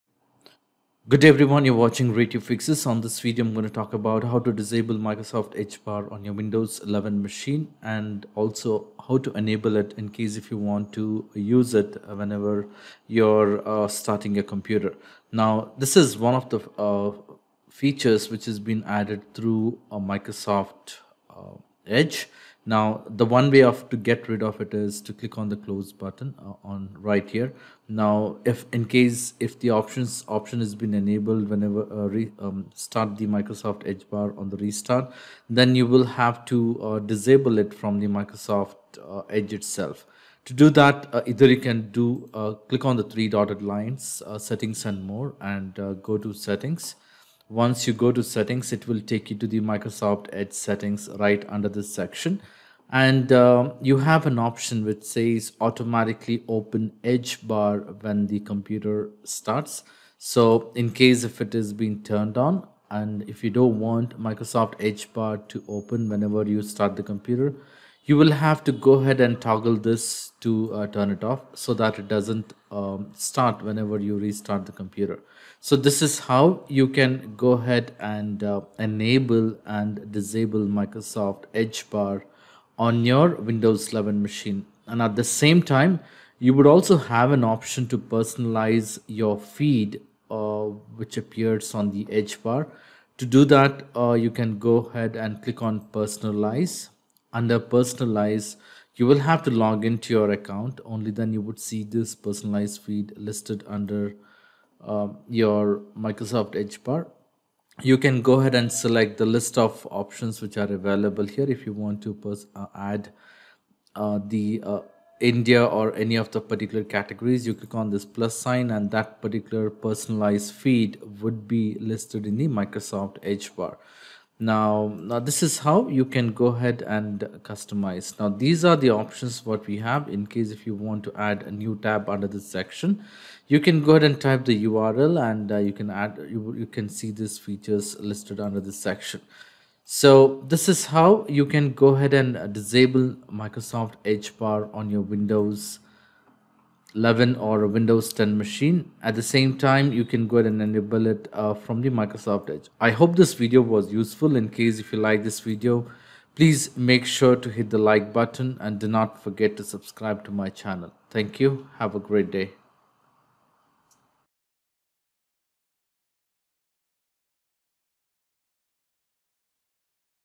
How to Enable and Disable Edge Bar in Windows 10 and Windows 11
What is Edge Bar?
Microsoft Edge Bar is a feature in the Edge browser which is basically a mini Edge browser that sits on the right side of your screen. This feature is similar to the Microsoft News and Weather widget but it’s more than that. It can work both as an extension of the Edge browser or an independent widget that displays content even when the browser is closed.